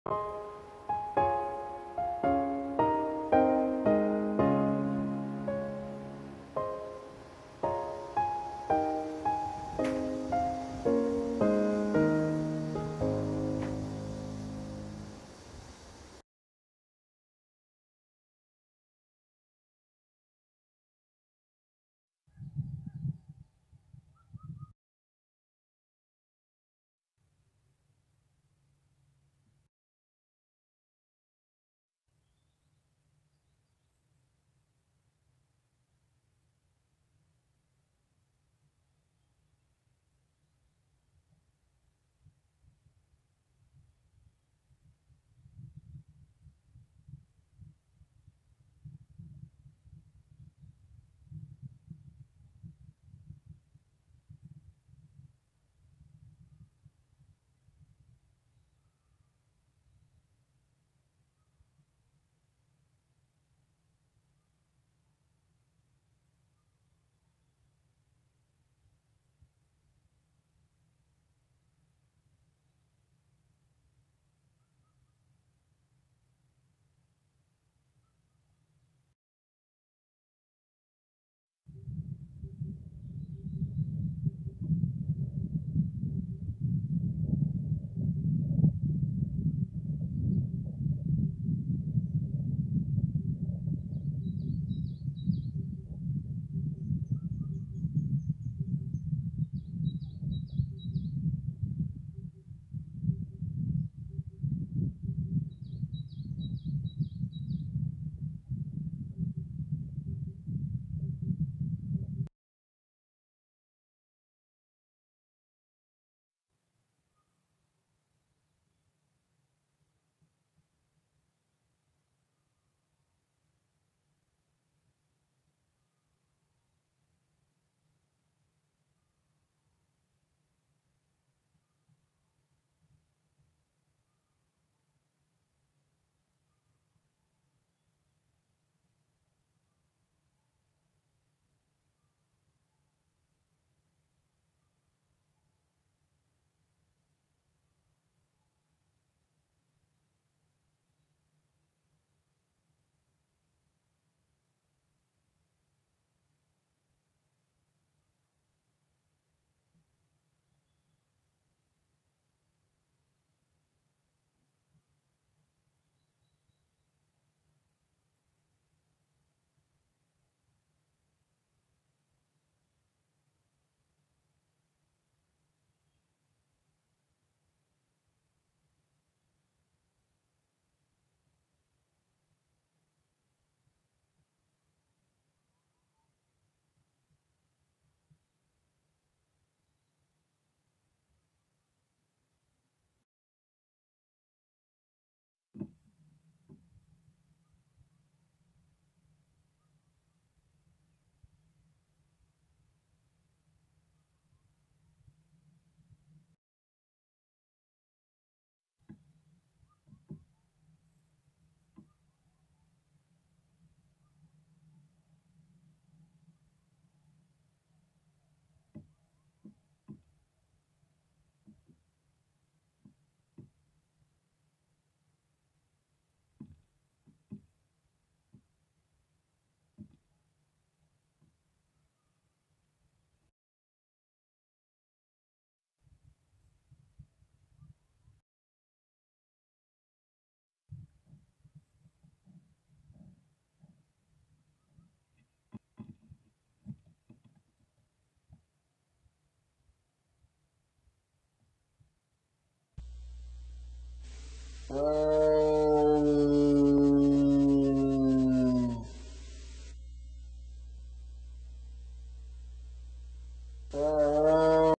임순남 Tiger Korea DMZ 2019.5/14 AM 6~PM7실시간 방송 #5 Korea DMZ Real time broadcasting #6
임순남 Korea DMZ 2019.5/14 AM 6~PM7실시간 방송 #5 Korea DMZ Real time broadcasting #6
동북아 한국 야생호랑이 서식,생태 ,자연, 다큐멘터리

Northeast Asia Korean wild tiger habitat, ecology, nature, documentary
(사)
KoreaDMZ 야생동식물생태과학원
(사)한국호랑이보호협회